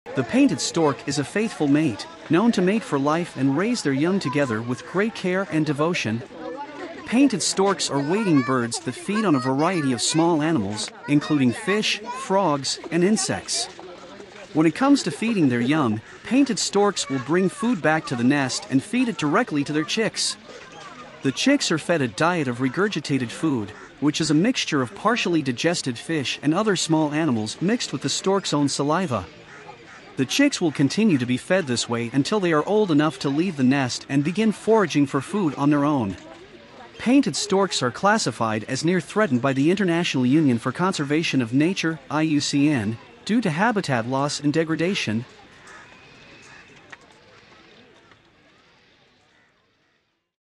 Parent Bird Feeding Live Fish to Chicks – Unbelievable Wildlife Footage!
The painted stork is a faithful mate, known to mate for life and raise their young together with great care and devotion. Painted storks are wading birds that feed on a variety of small animals, including fish, frogs, and insects. When it comes to feeding their young, painted storks will bring food back to the nest and feed it directly to their chicks. The chicks are fed a diet of regurgitated food, which is a mixture of partially digested fish and other small animals mixed with the stork's own saliva. The chicks will continue to be fed this way until they are old enough to leave the nest and begin foraging for food on their own. Painted storks are classified as "near threatened" by the International Union for Conservation of Nature (IUCN) due to habitat loss and degradation.